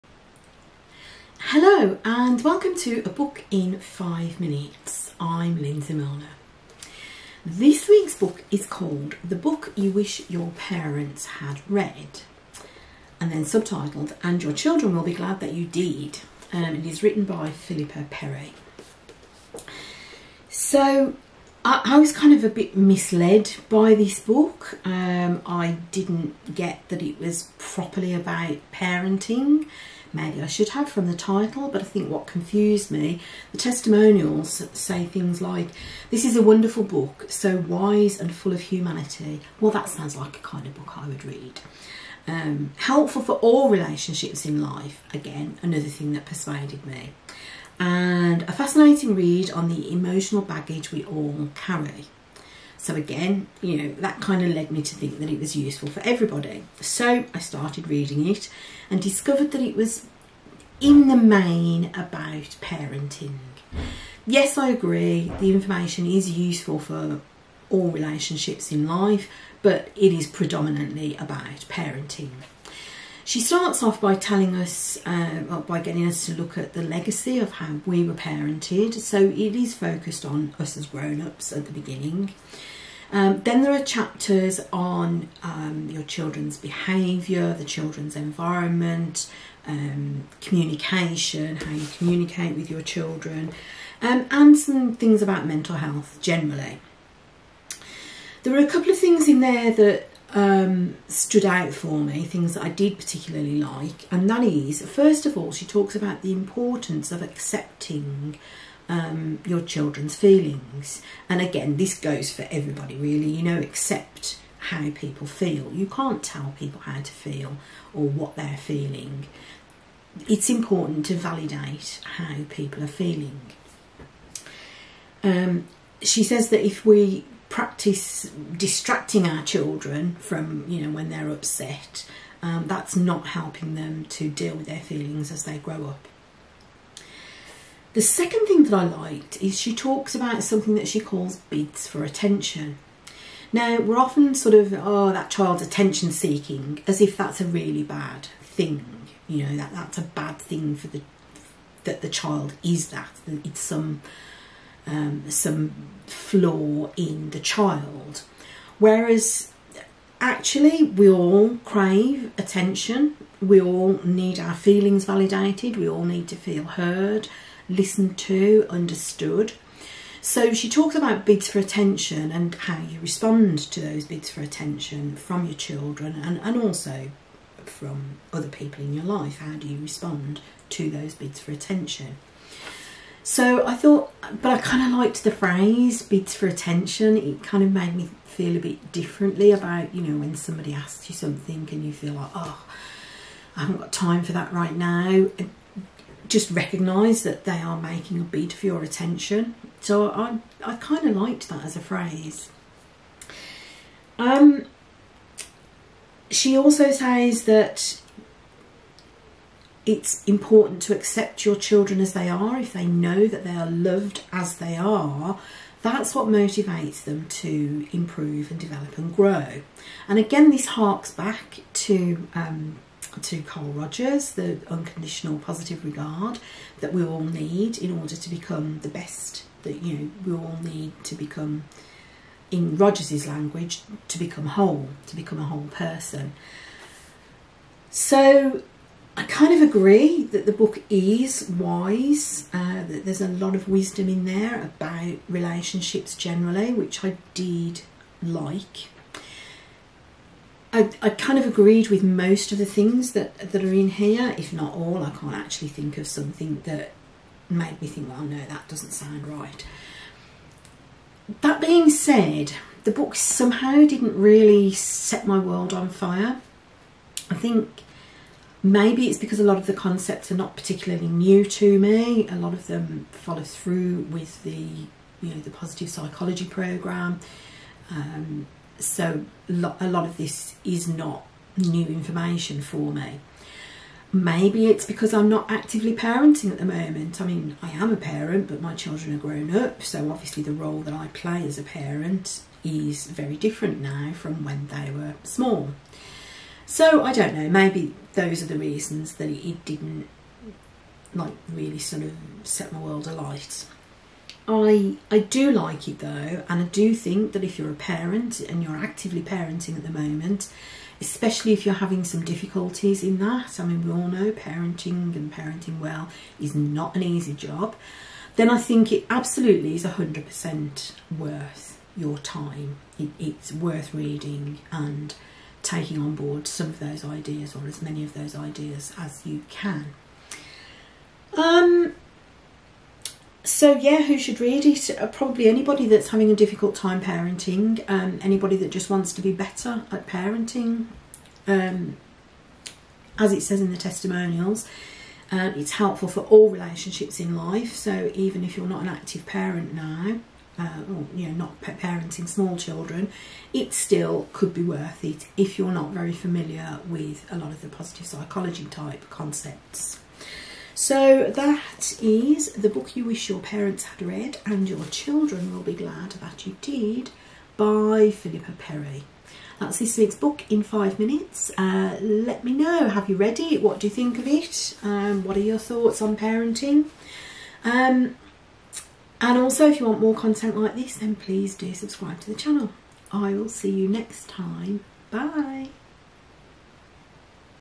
A book in five minutes The book you wish your parents had read
Book review
The book you wish your parents had read (and your children will be glad that you did)
by Philippa Perry